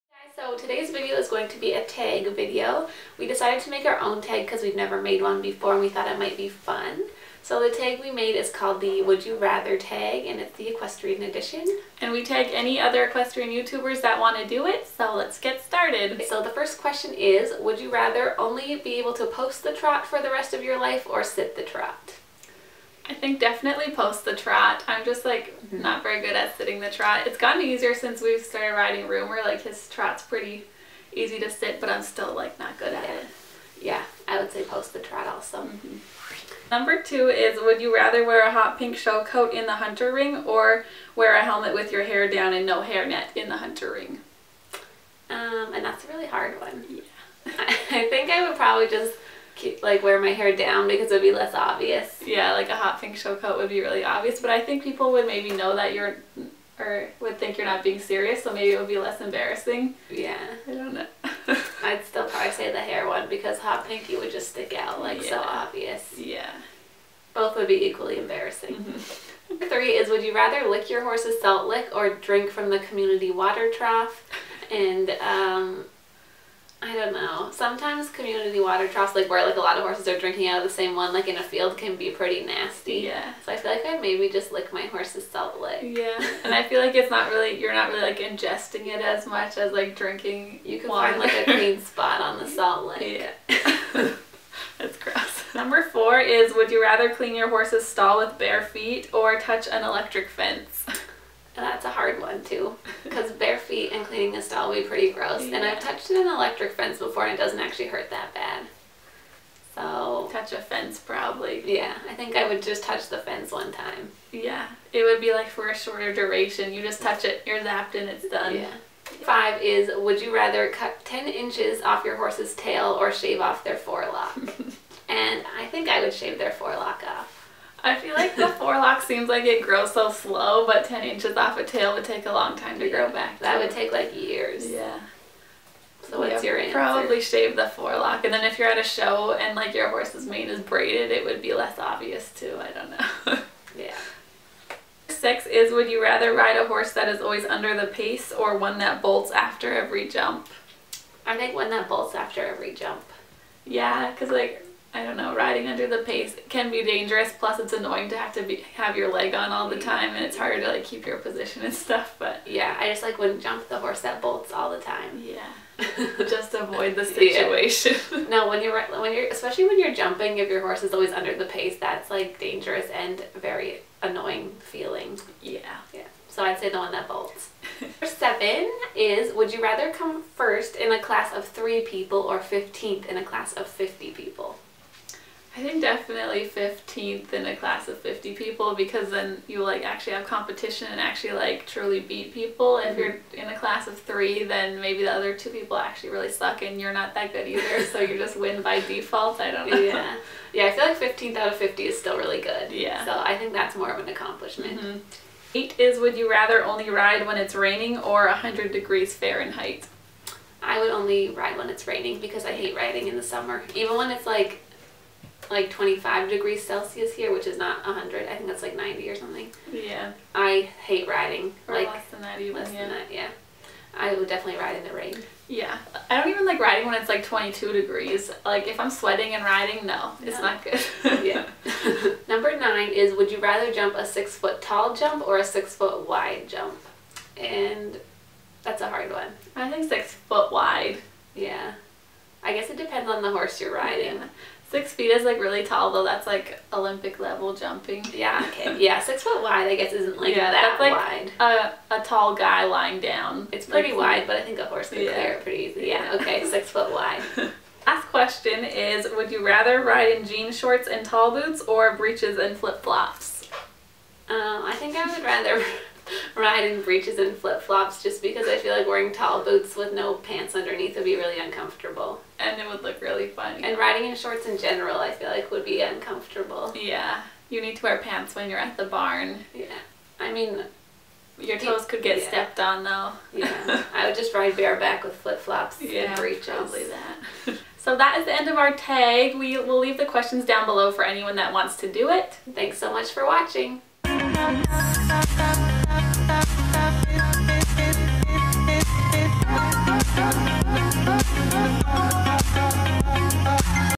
WOULD YOU RATHER TAG | EQUESTRIAN EDITION
MAKE YOUR OWN TAG VIDEO :D
Would you rather:
1. Only be able to post the trot or sit the trot for the rest of your life.
2. Show in the hunter ring in a hot pink show coat or with your hair down and no hair net
3. Lick your horse's salt lick or drink from the community water trough
4. Clean your horse's stall with bare feet or touch an electric fence
5. Cut 10 inches off your horse's tail or shave off their forelock
6. Ride a horse that is always under the pace or one that bolts after every jump
7. Come 1st in a class of 3 riders or 15th in a class of 50 riders
8. Only ride when it's raining or 100 degrees fahrenheit
9. Jump a 6 foot tall jump or a 6 foot wide jump
10. Ride in jean shorts and tall boots or breeches and flip flops